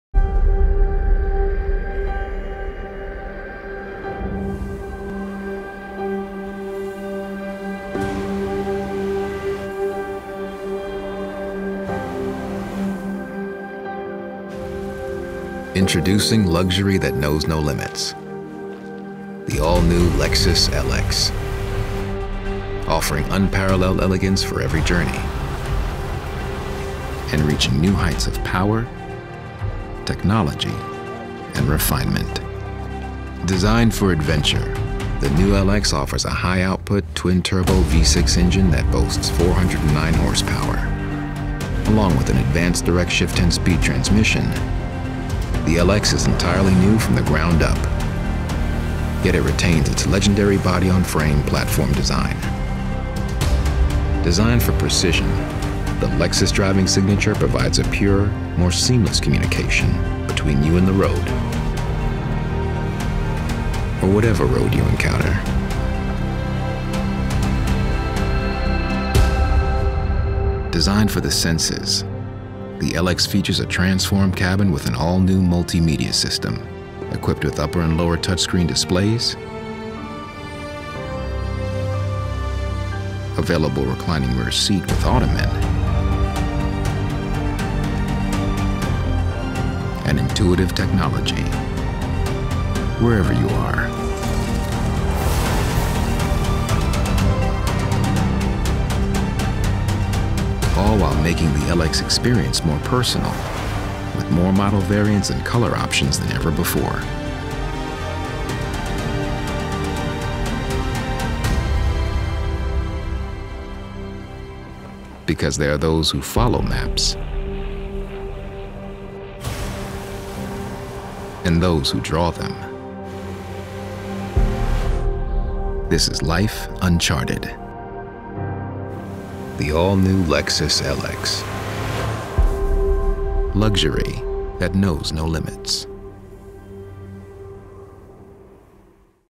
Introducing the New Lexus LX SUV
SOPHISTICATION, POWER & PRESENCE
The next-generation Lexus LX flagship SUV will provide the luxury of choice. New engine options, a new 10-speed automatic transmission, enhanced driver-aid technologies and the incorporation of the Lexus Driving Signature philosophy provide exceptional on and off-road capabilities.
The new LX range has expanded to include a new four-seat VIP grade for the LX 600 and the introduction of five-seat F Sport variants.